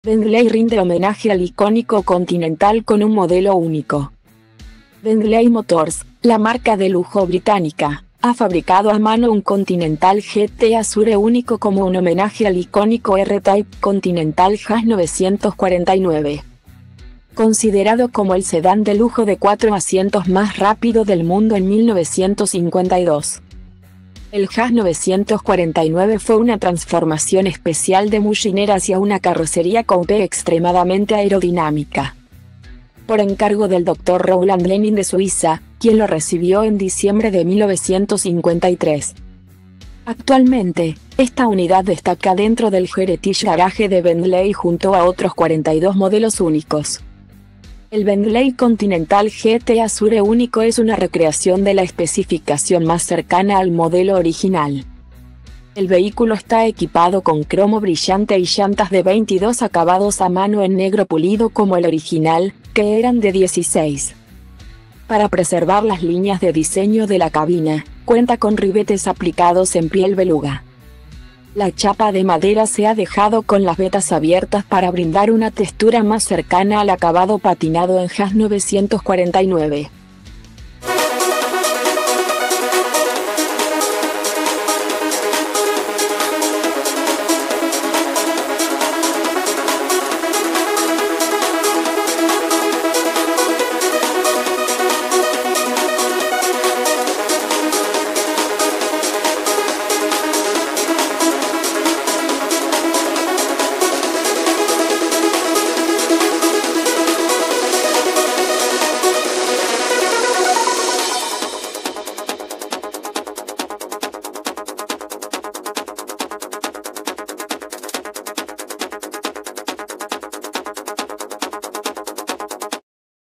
Bentley rinde homenaje al icónico Continental con un modelo únicoAr5
Bentley rinde homenaje al icónico  Continental con un modelo único
Bentley Motors, la marca de lujo británica, ha fabricado a mano un Continental GT Azure único como un homenaje al icónico R-Type Continental JAS 949,considerado como el sedán de lujo de cuatro asientos más rápido del mundo en 1952. El JAS 949 fue una transformación especial de Mulliner hacia una carrocería coupé extremadamente aerodinámica,